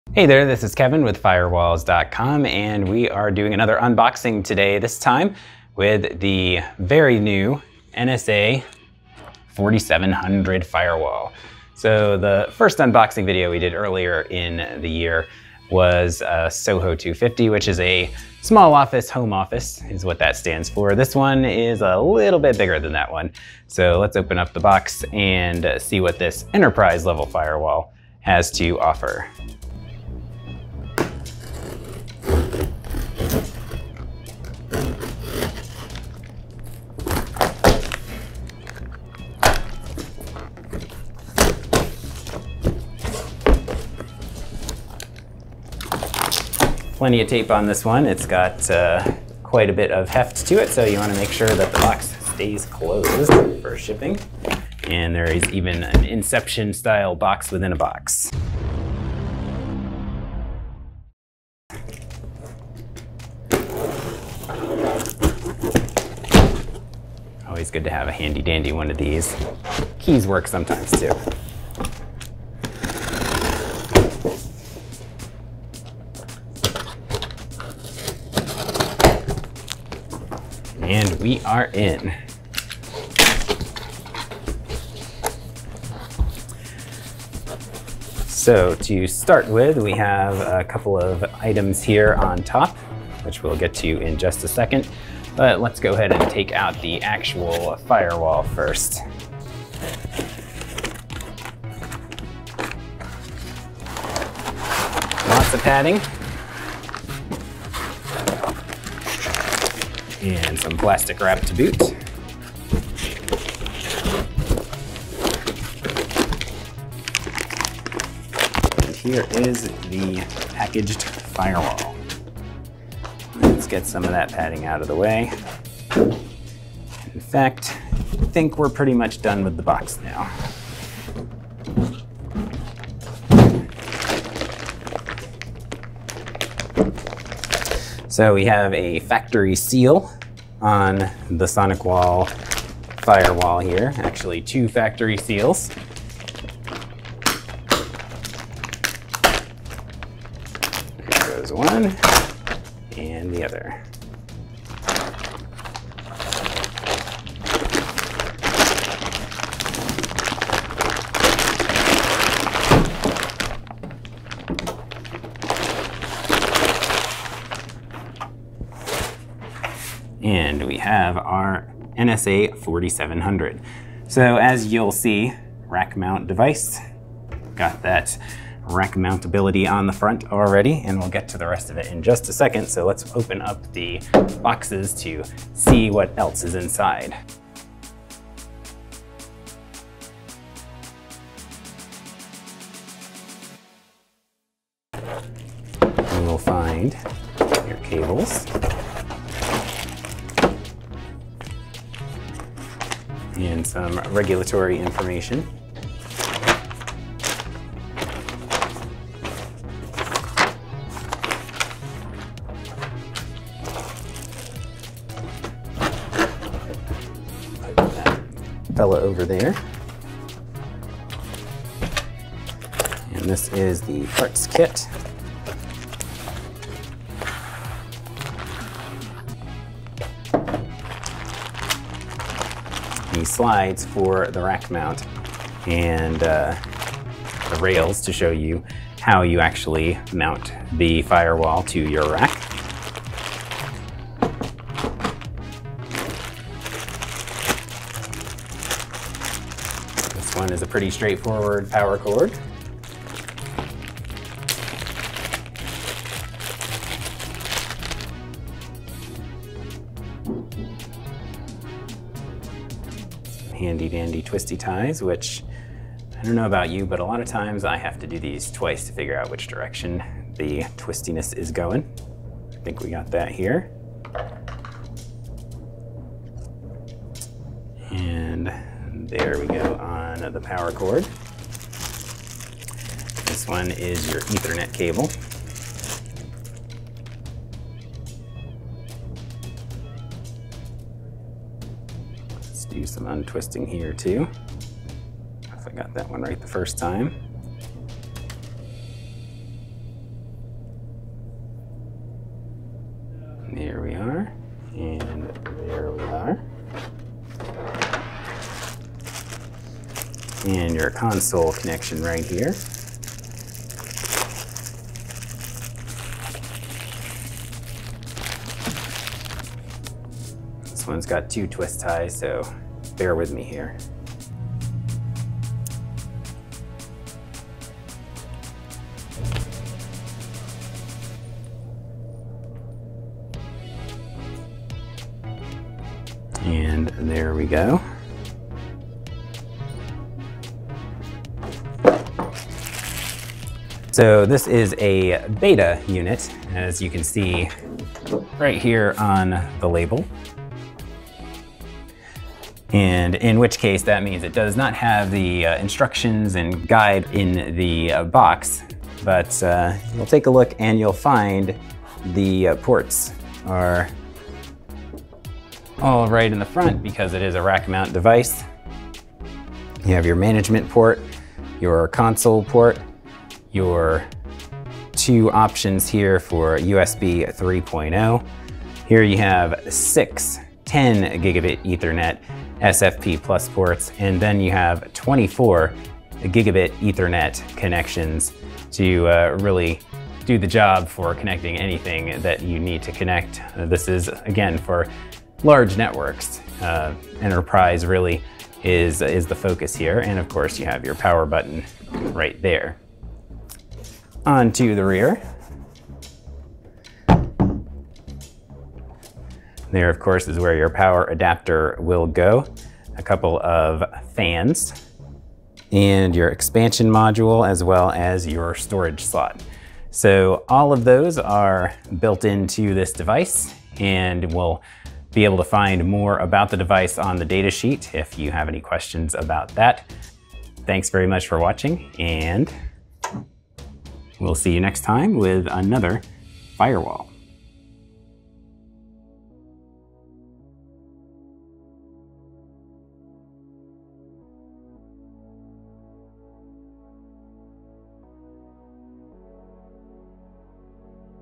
Firewall Unboxing: SonicWall NSa 4700 Firewall
When you're unboxing firewalls, what can you expect to find when the device is delivered?

In the second 2021 edition of our firewall unboxing series, we take a look at what's inside the box for the SonicWall NSa 4700 firewall. Learn all the contents of the NSa 4700 box, and see first-hand what this new this summer enterprise firewall looks like as one of the flagships of SonicWall's Gen 7 firewall series.

Oh, and have a firewall that you'd like to see unboxed? Drop an idea for a future firewall unboxing video in the comments!